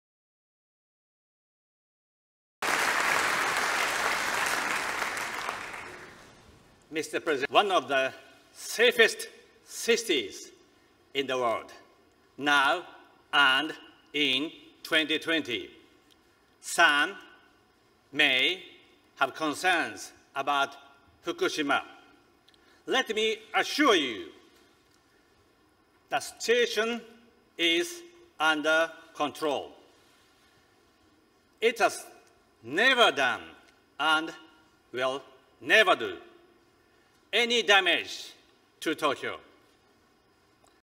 【「アンダーコントロールは官僚が書いたプロンプター」と安倍は責任回避】下手な英語読み上げのプロンプター scratch 小学生 政治に立ち上がる
引用：平成25年9月7日ＩＯＣ総会